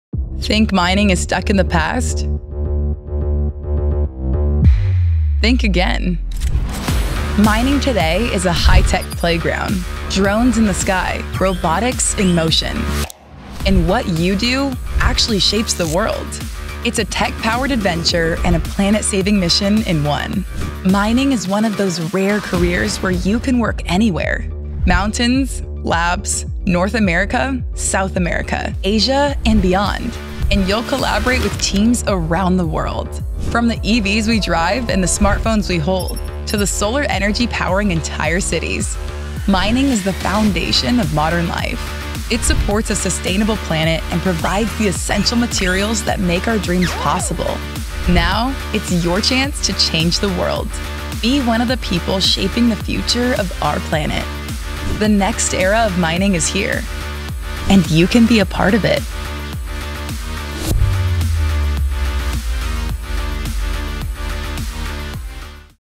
A Career That Actually Matters!
Mining isn’t stuck in the past — it’s shaping the future.
Think drones, robotics, global teams, and tech that powers EVs, smartphones, and clean energy. 🌍⚡
This is where innovation meets impact — and where your work can help build a more sustainable planet.

The next era of mining is here. Be part of it.